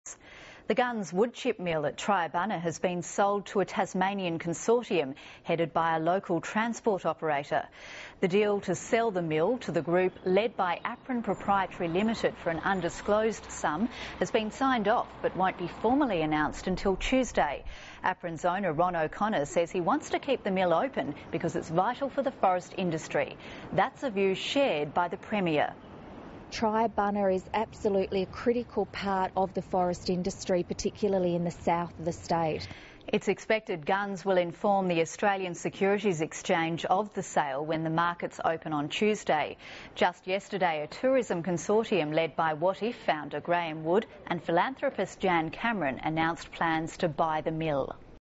Gunns mill sold to local group
The Triabunna Gunns woodchip mill on the south-east coast of Tasmania has been sold to a consortium headed by a local transport operator.